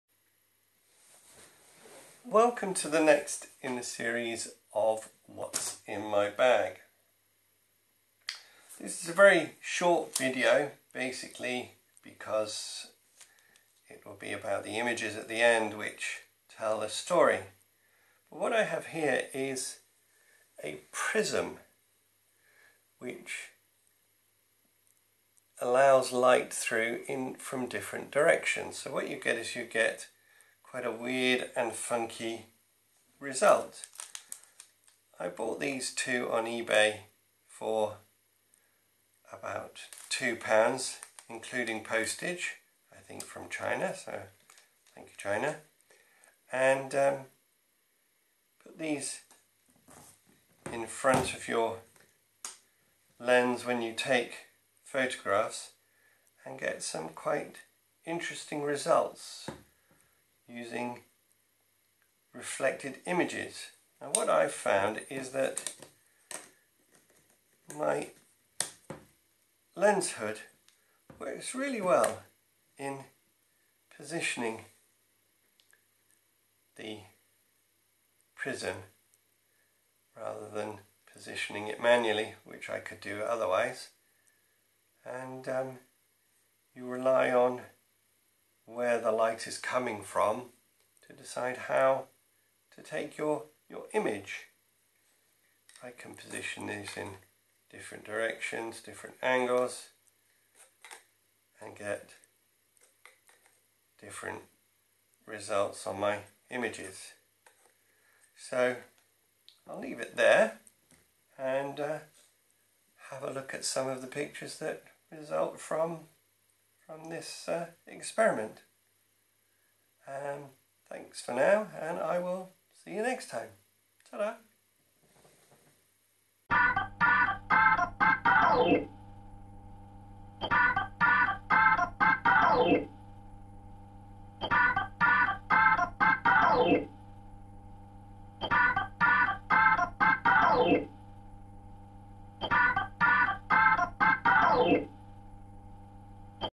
DIY Prism lens filter for DSLR
Here’s a cheap DLSR filter that can be used to add an interesting effect… a prism. I got a couple of small rectangular prisms for next to nothing… see what they can do.
Have fun with this DIY idea.